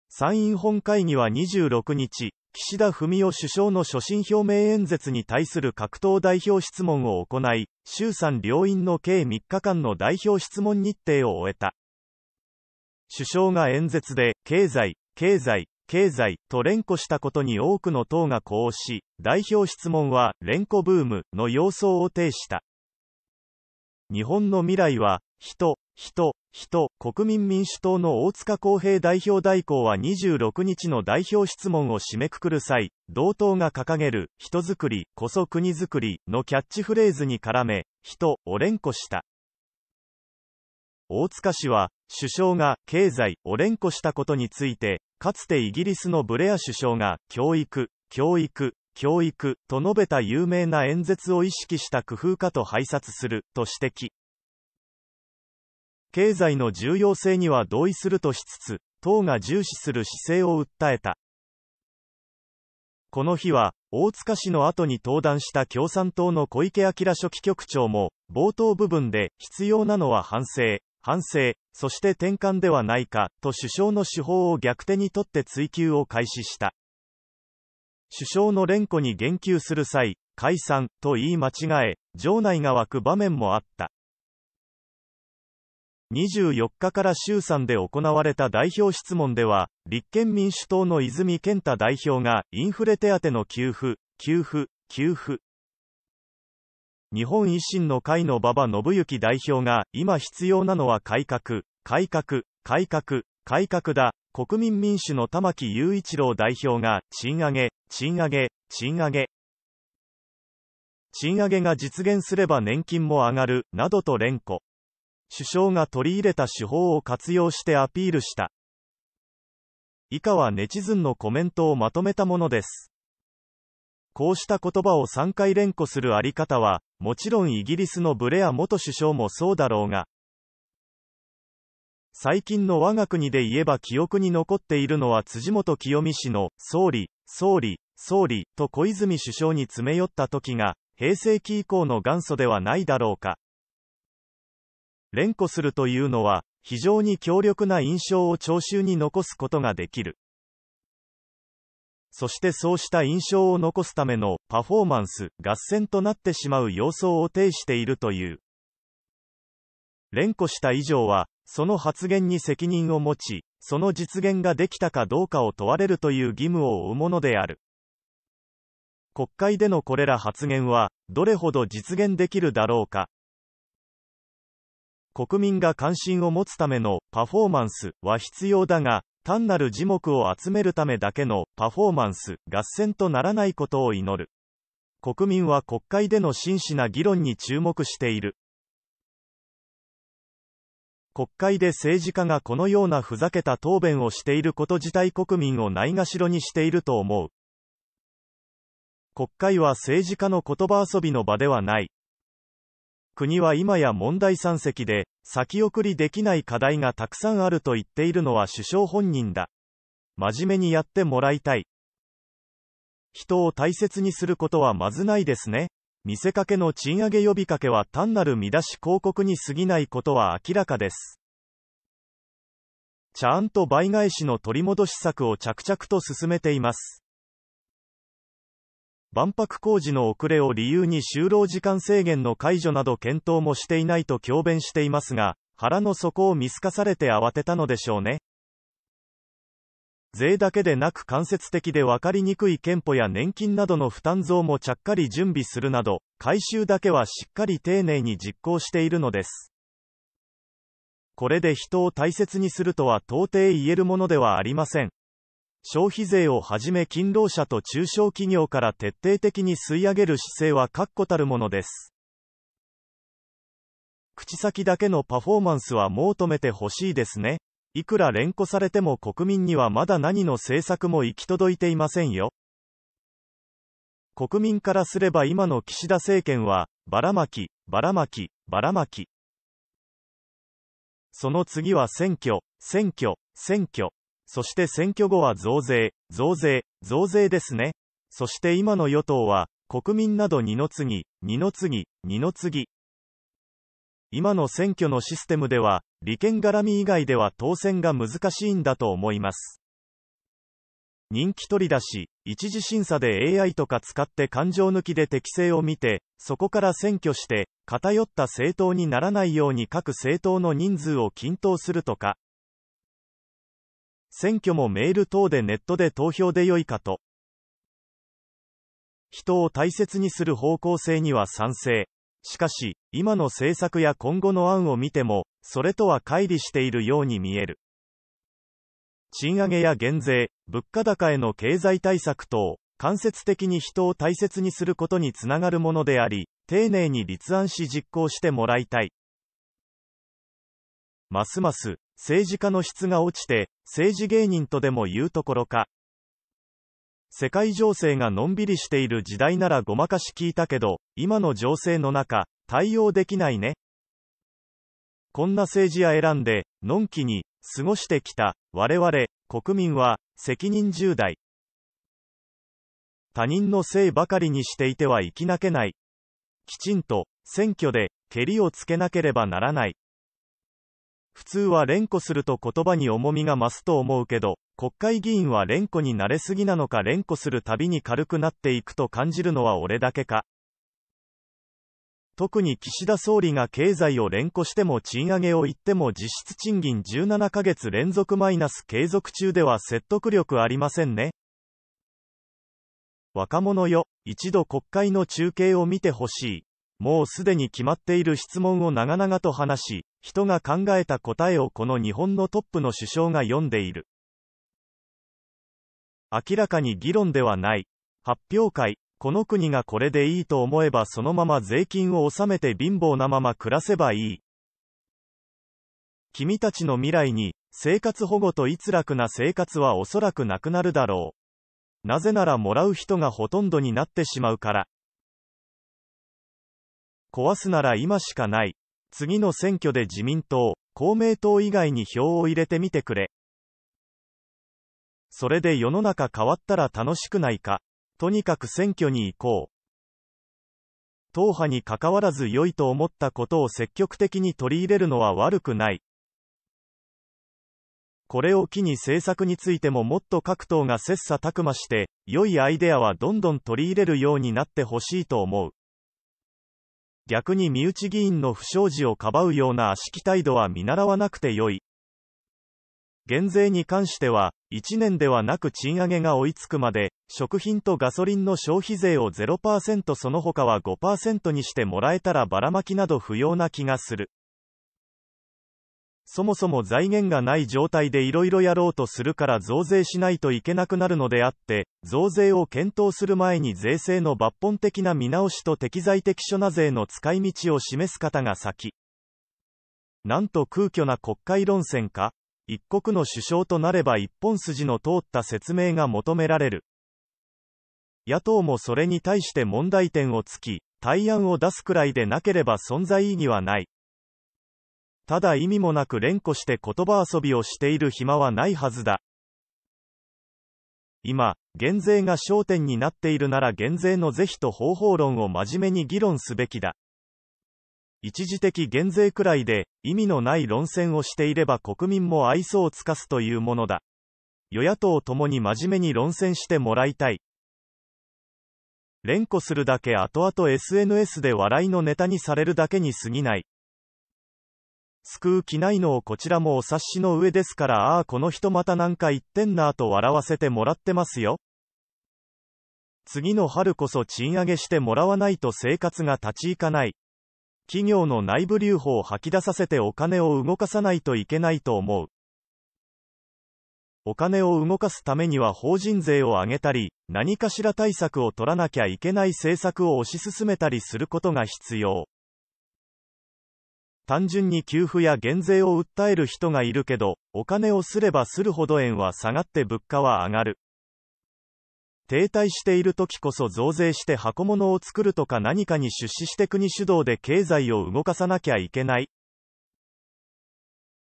「人、人、人」「改革、改革、改革」国会で連呼ブーム 首相に呼応 国会で政治家がこのようなふざけた答弁をしていること自体国民を蔑ろにしていると思う。
首相の連呼に言及する際、「解散」と言い間違え、場内が沸く場面もあった。